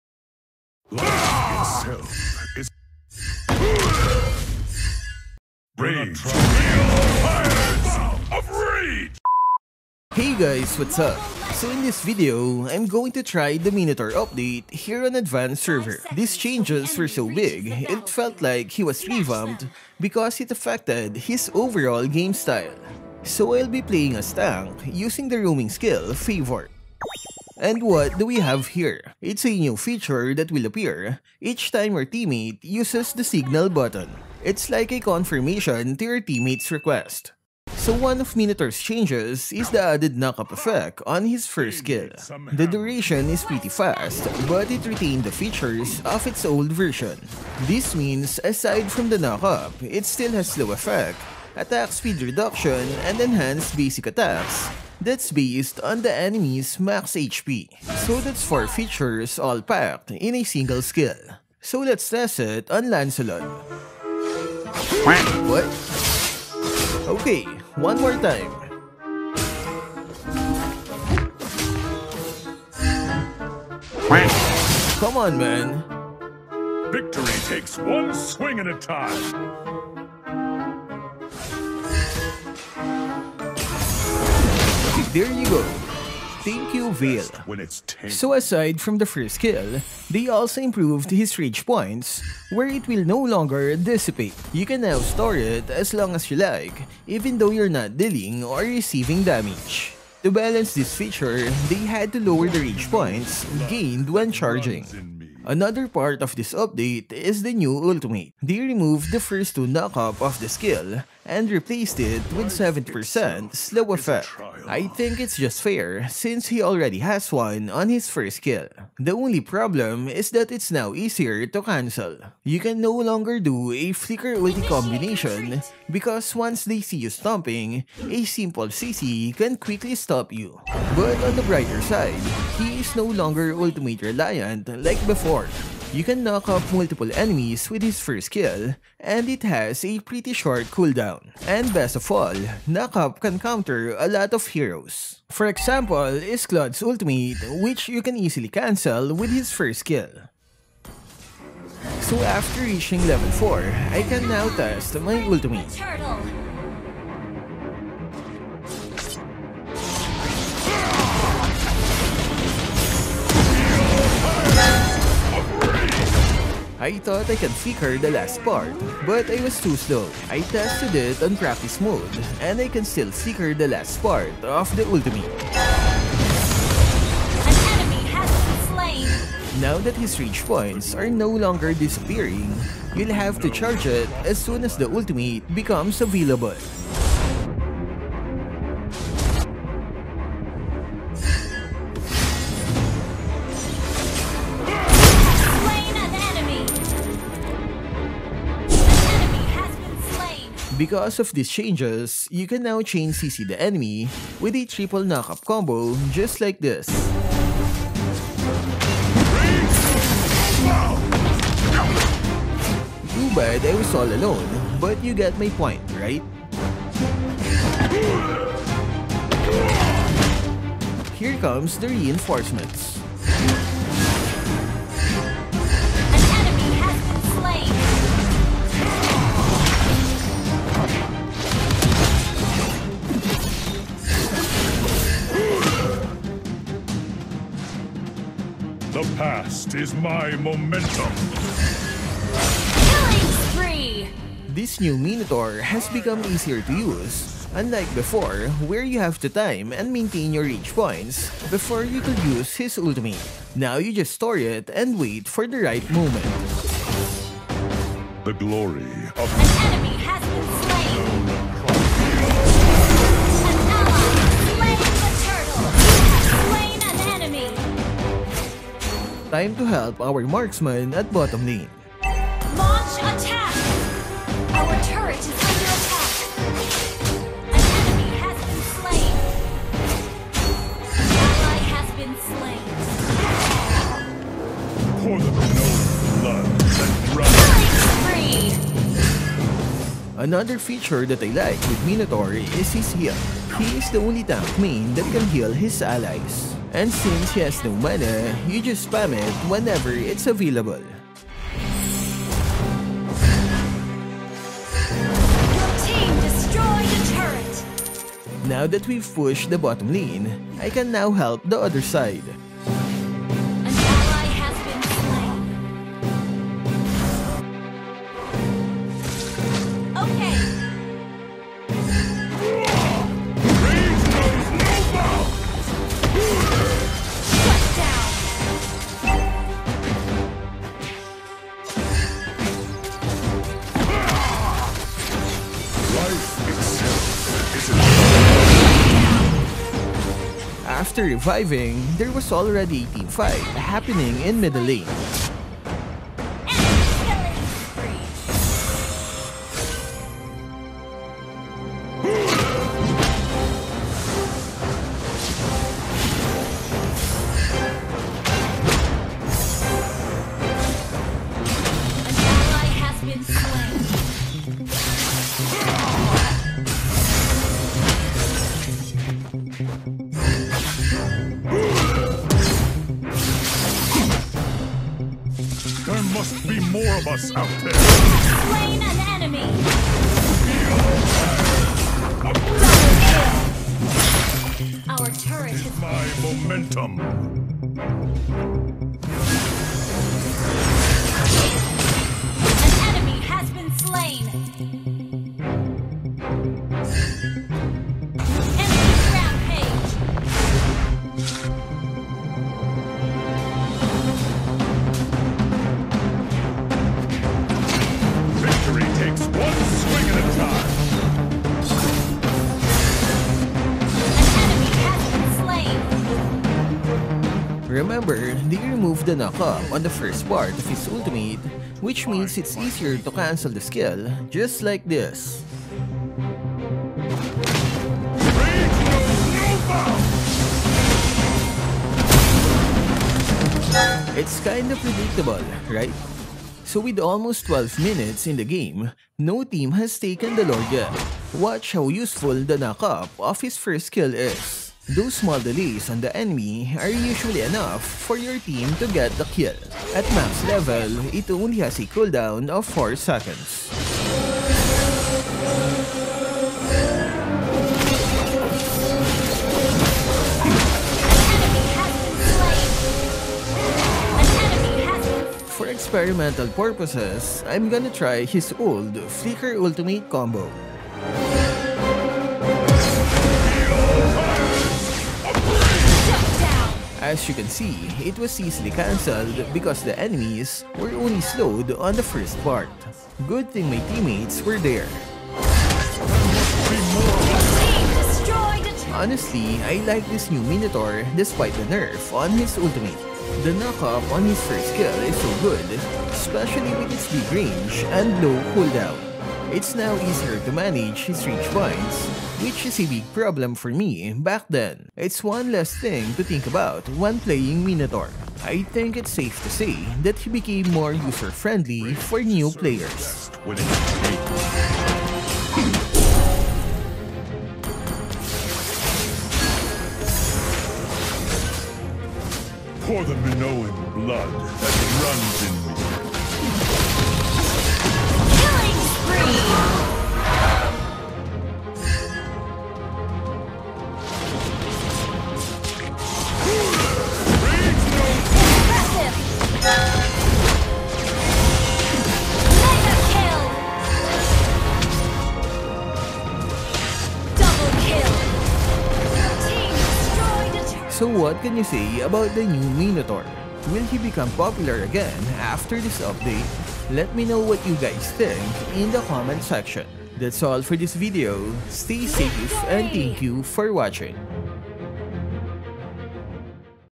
MINOTAUR WITH A NEW SPAMMABLE KNOCK-UP SKILL
So in this video I'm going to try the Minotaur update here on advance server
These changes were so big it felt like he was revamped because it affected his overall game style